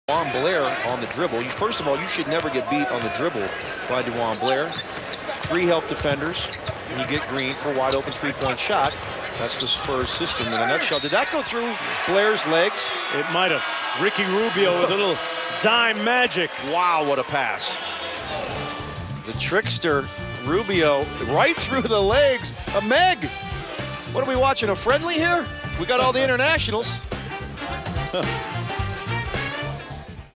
Ricky Rubio Dishes Between the Legs of the Defense
Ricky Rubio serves up the dish through DeJuan Blair's legs to find Greg Stiemsma for the layup plus the foul.  for more highlights.

Ricky Rubio asiste entre las piernas de DeJuan Blair hacia Greg Stiemsma para la marca con la falta.  Visite para mas espectaculos.